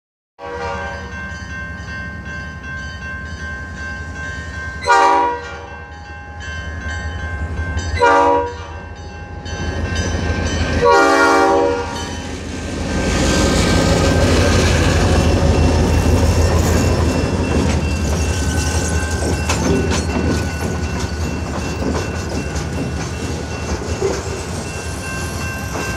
ELS Train Suamico
ELS Train going to Green Bay to do switching.